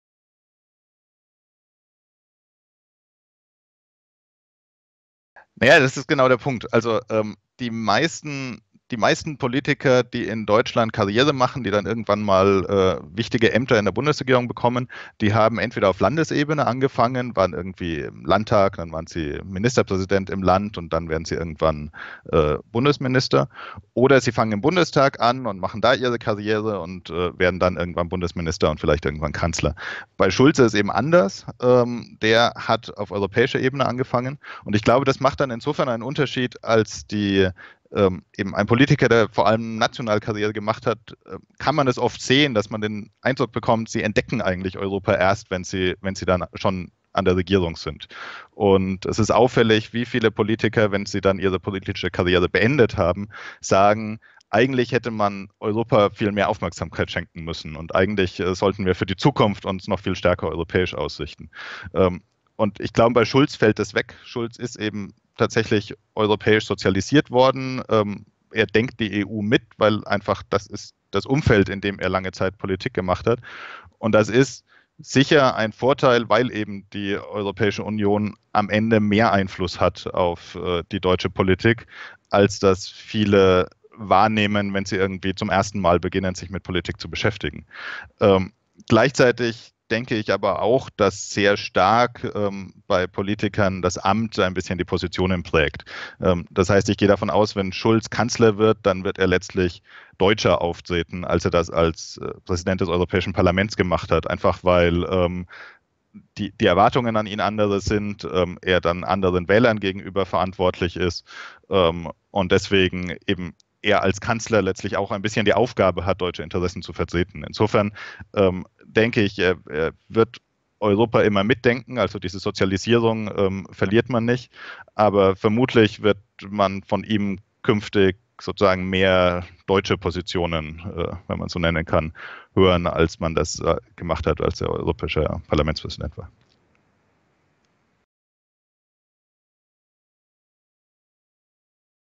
Manuel Müller über Martin Schulz und Europa
Der EU-Blogger Manuel Müller beantwortet Schülerfragen der Schwarzkopf-Stiftung zu Martin Schulz (SPD).